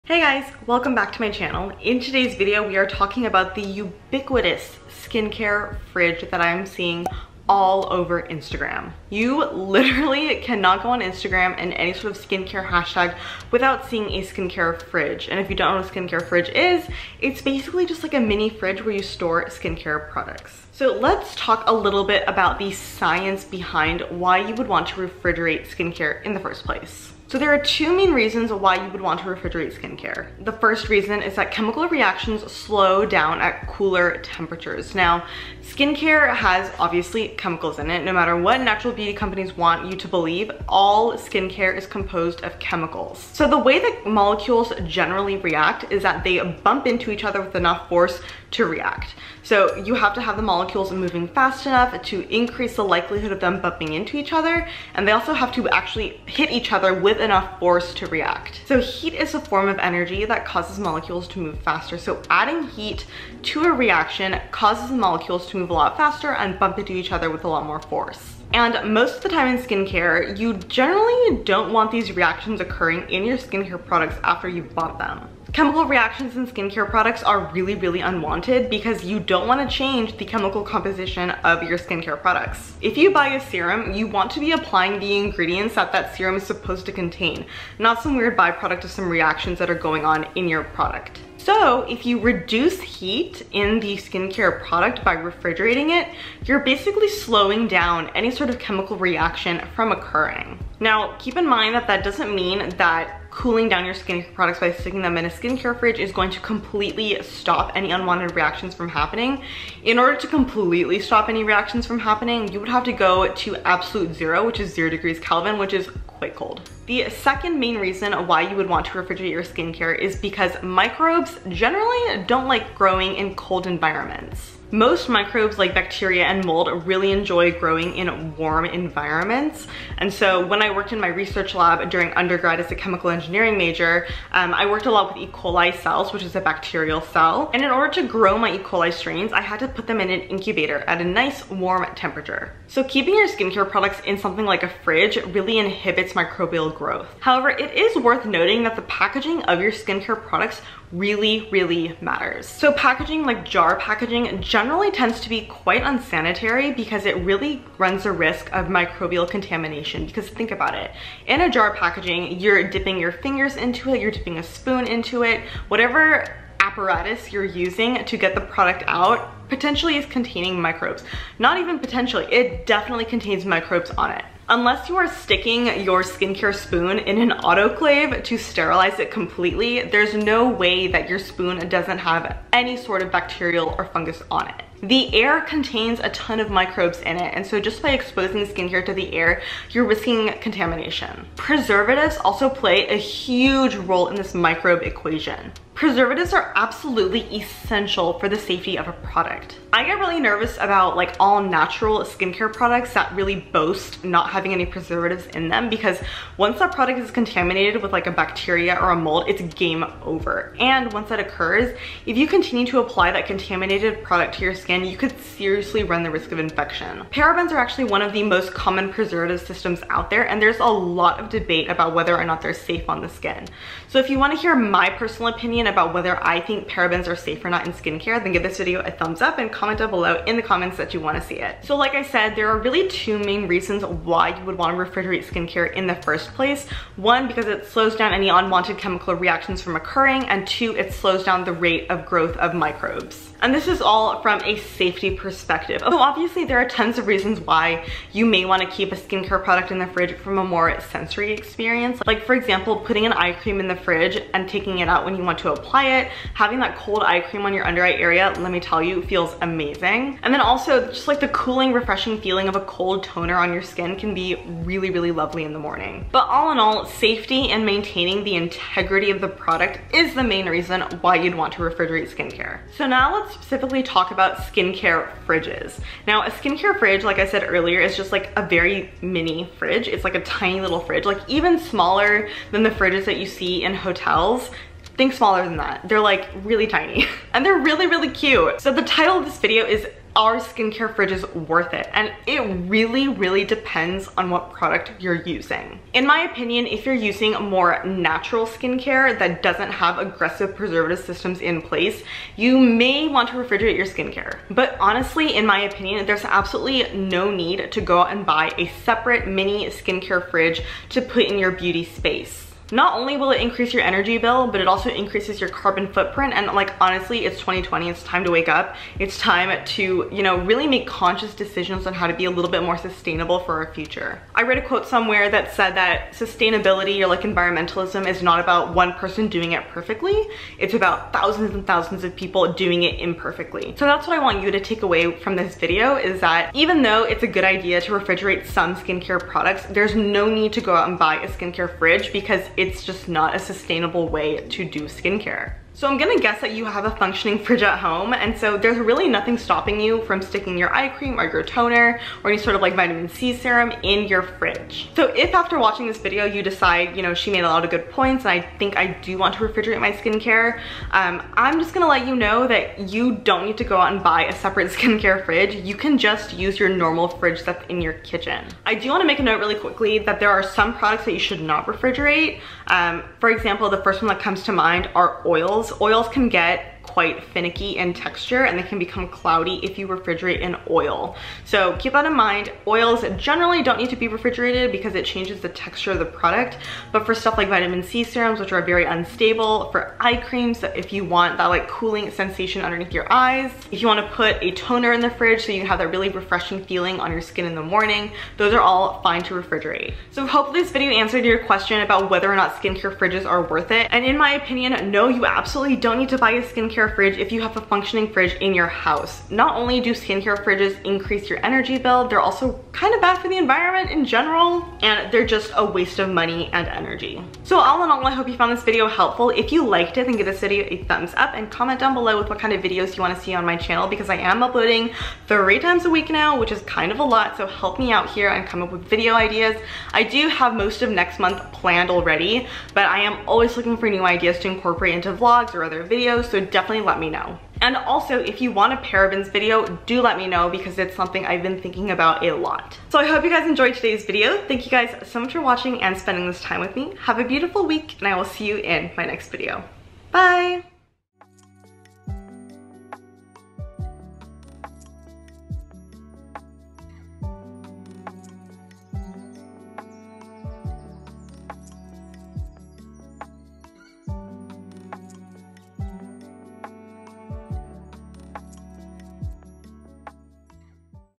ARE SKINCARE FRIDGES WORTH IT? The Science Behind Refrigerating Skincare | KAYA EMPIRE
Today's video discusses the chemistry and microbiology behind refrigerating skincare beauty products and keeping them nice and cold to prevent spoilage and microbial contamination. I also talk about the environmental impact of skincare fridges! Do we agree?
 Give this video a thumbs up for a video about PARABENS!

Products:
Example of a Skincare Fridge

*PR Gifted